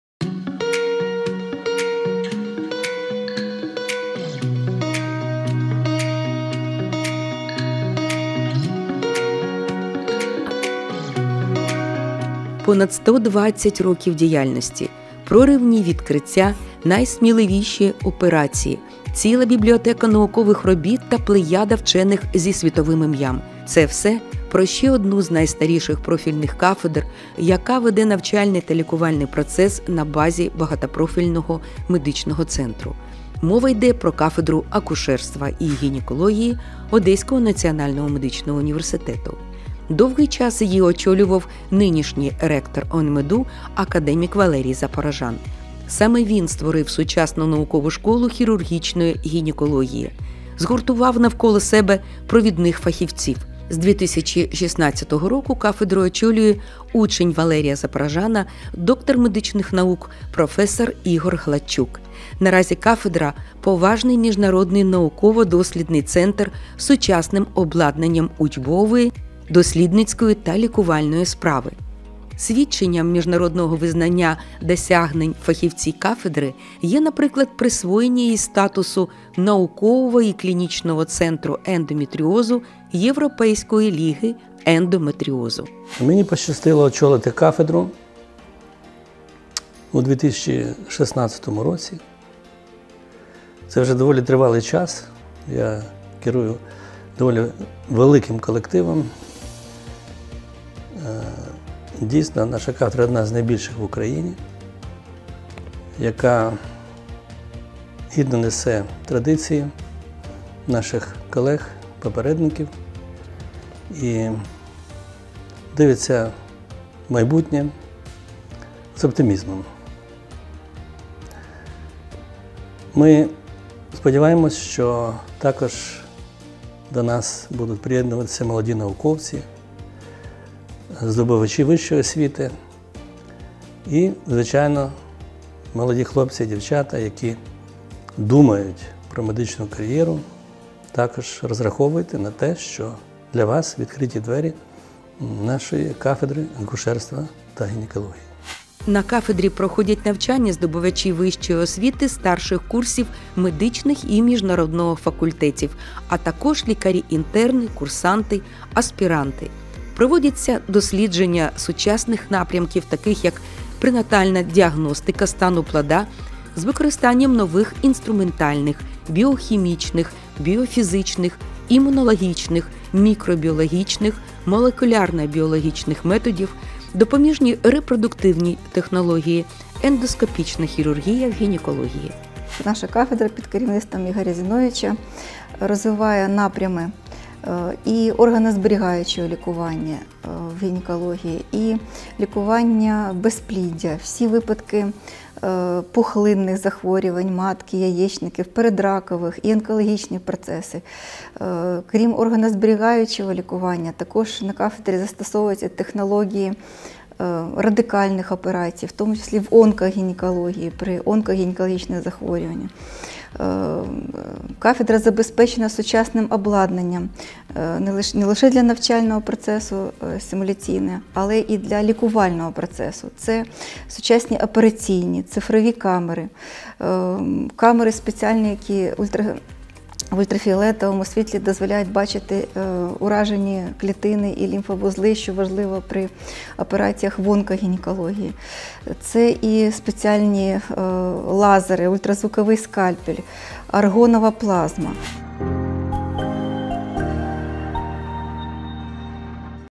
Ваше здоров'я у надійних руках! Кафедра акушерства і гінекології ОНМедУ
Понад 120 років діяльності, проривні відкриття, найсміливіші операції, ціла бібліотека наукових робіт та плеяда вчених зі світовими іменами – це все про ще одну з найстаріших профільних кафедр, яка веде навчальний та лікувальний процес на базі БМЦ. Мова йде про кафедру акушерства і гінекології Одеського національного медичного університету.